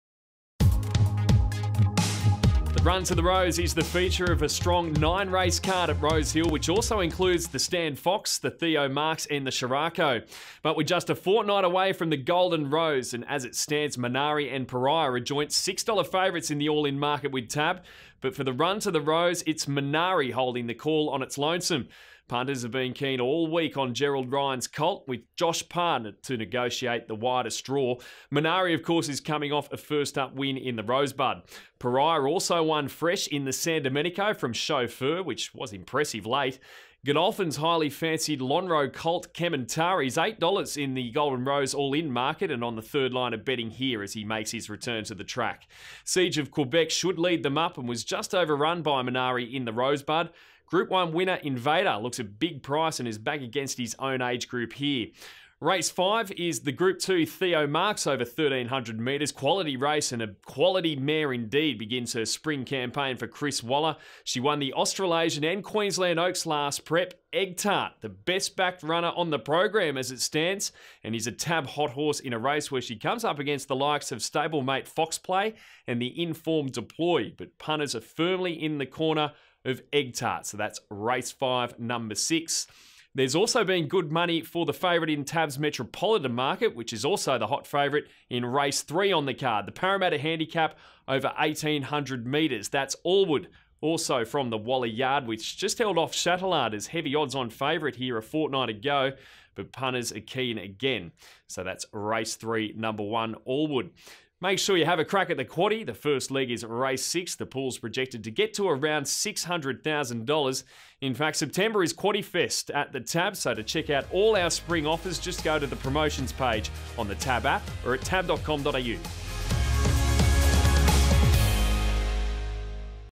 TAB Market Update - 9 September 2017
Gerard Middleton from TAB gives us a market update ahead of the first weekend of the 2017 Sydney Spring Carnival; Run to the Rose Day, 9 September 2017, at Rosehill Gardens.